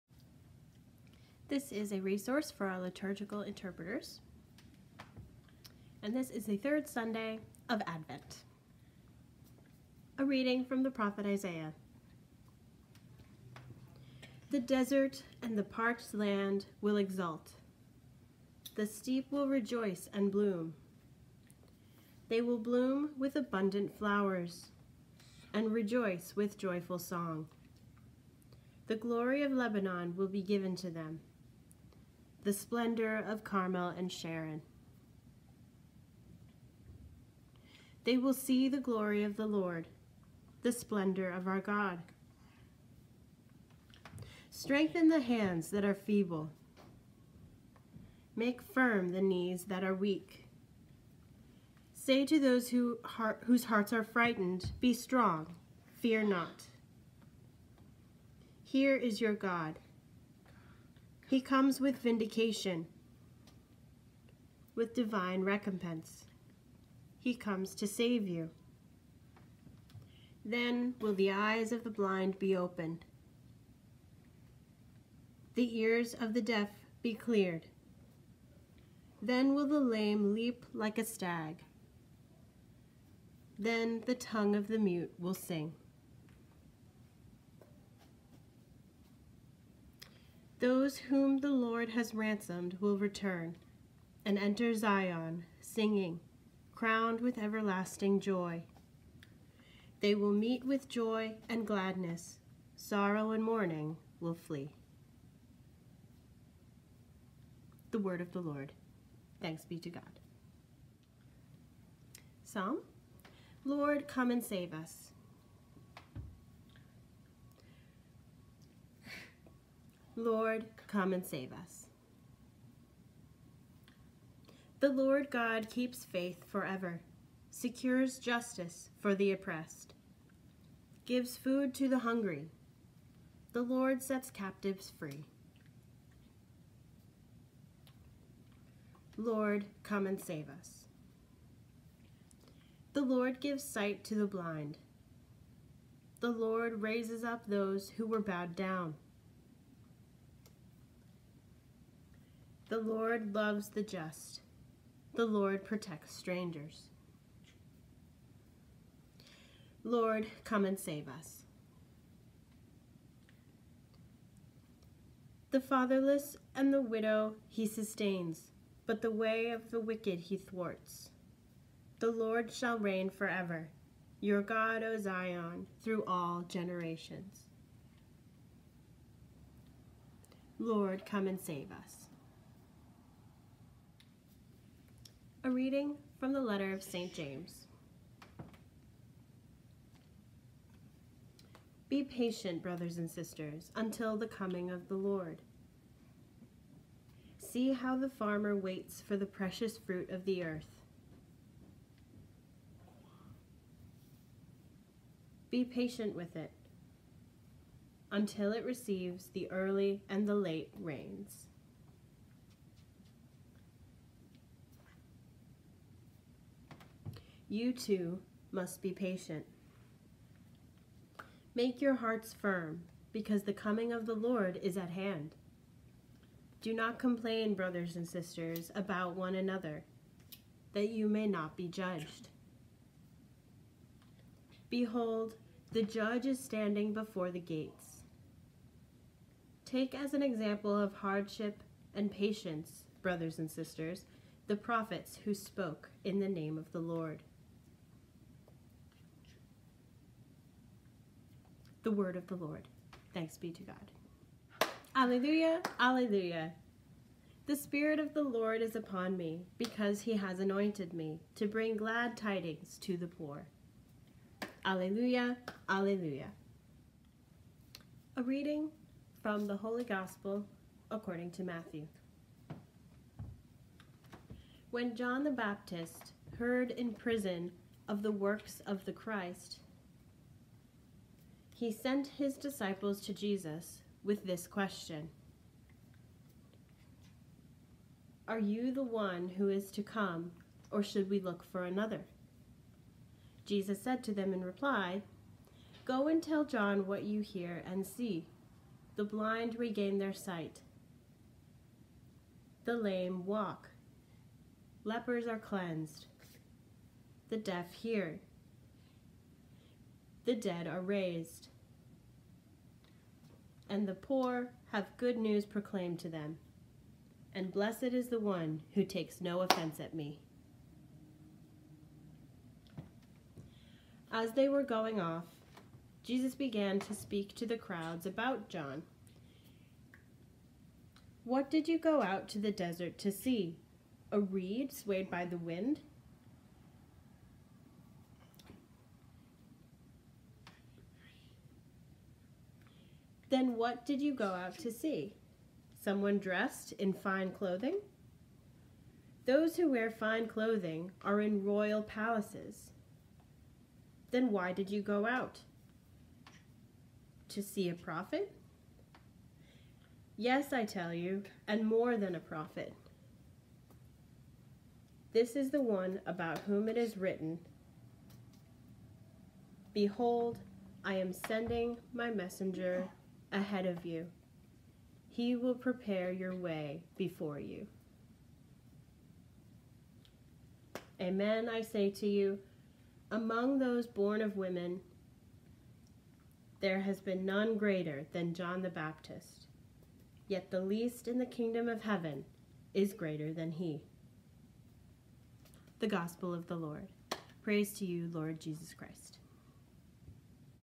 Handing on the Gospel 3rd Sunday Advent Year A
This is our resource for Liturgical ASL Interpreters for the upcoming Sunday Mass. Our videos feature Deaf signers for all of the readings so that our interpreters can see a Deaf ASL model to inform their linguistic decisions on Sunday.